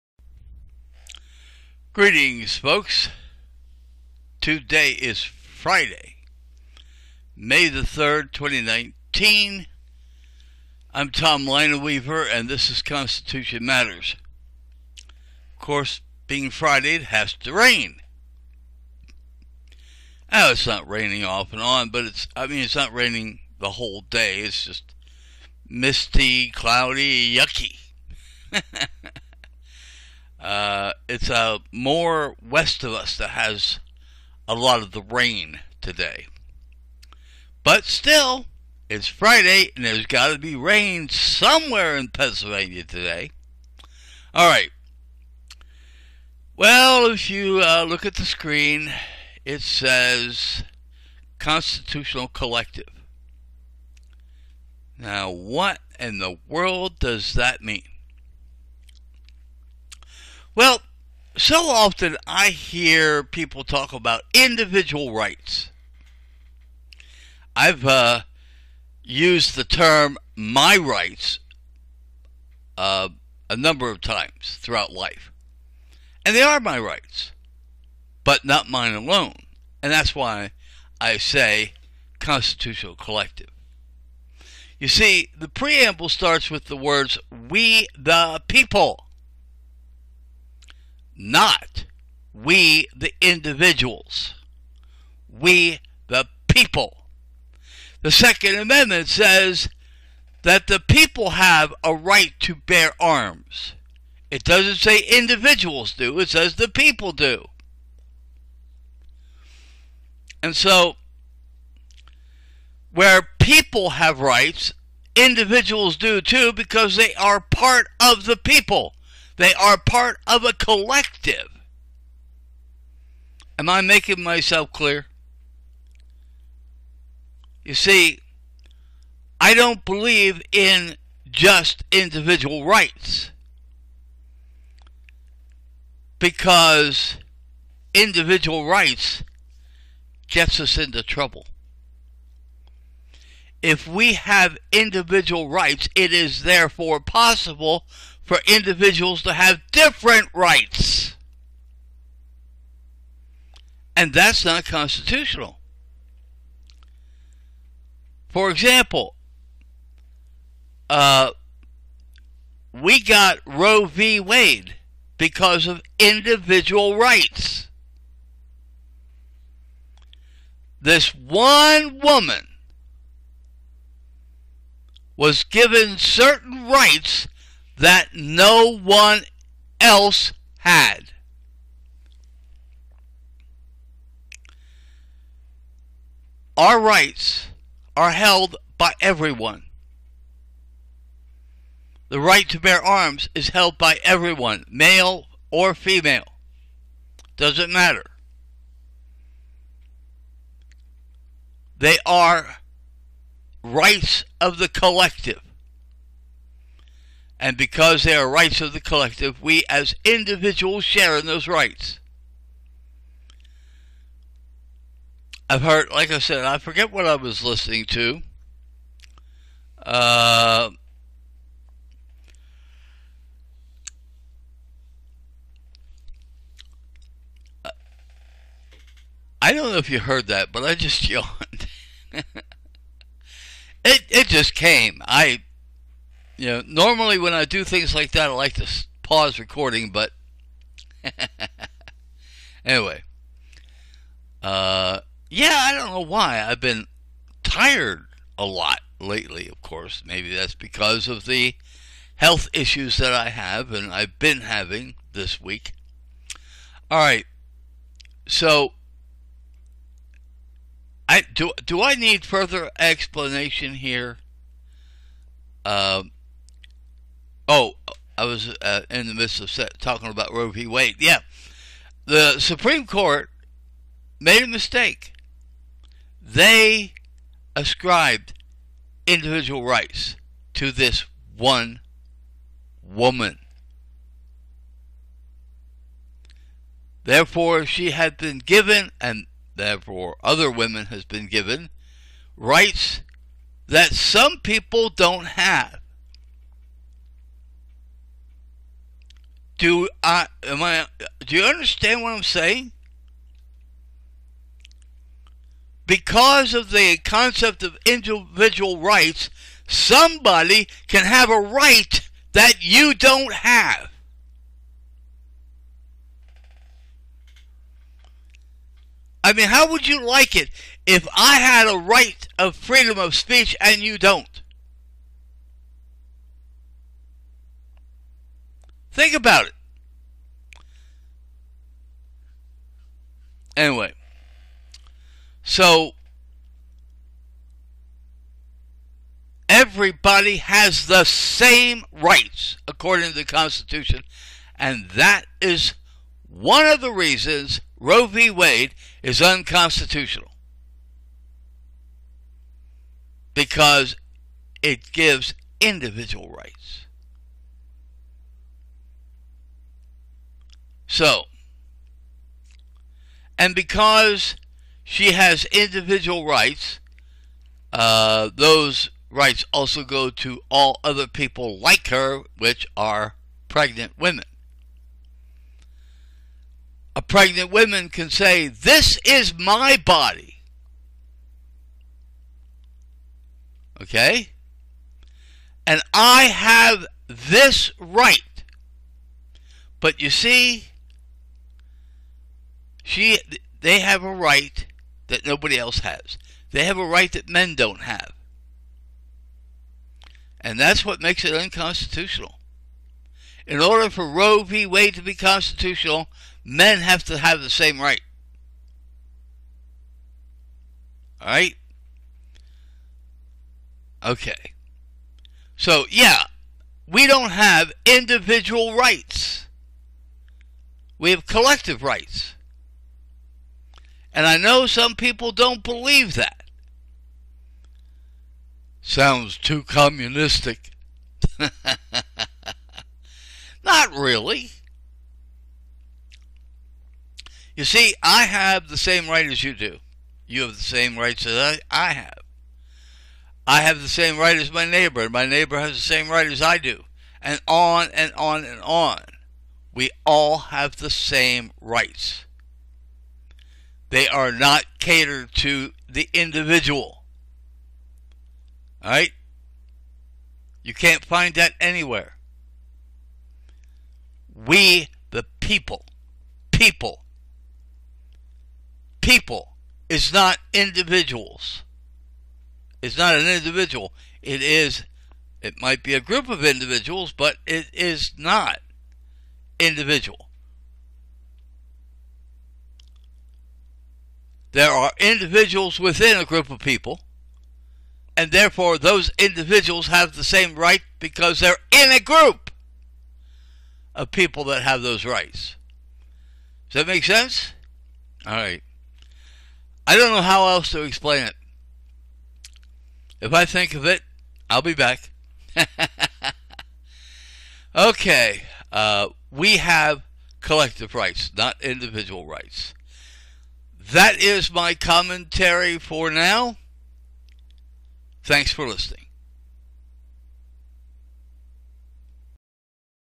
Constitutional Collective
The Constitution gives collective rights, not individual.